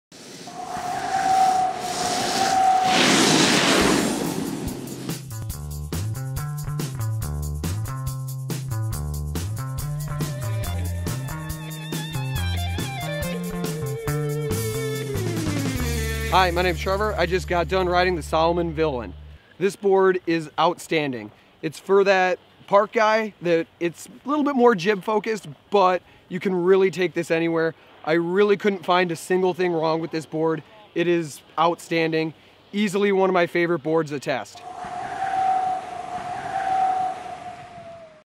Trevor's Review-Salomon The Villain Snowboard 2019-Snowboards.com
While most villains are the enemy of the hero, The Villain by Salomon is both a hero on the mountain and an enemy to the competition. With Salomons Aspen SLCT + Triax core and Rock Out Camber profile this board is playful in the terrain park and confidence inspiring on the groomers. The true twin shape offers easy transitioning for riding switch and the Quadralizer sidecut invites high speed carving with superior edge hold and control. Bringing it all together is the Popster Booster construction with carbon stringers that add even more pop then the standard popster. All those features plus the amazing art on top and bottom and it becomes clear why this board is considered both a hero and a villain, depending on who you are asking. If you want a great park board with superior design and technology than look no further than The Villain by Salomon.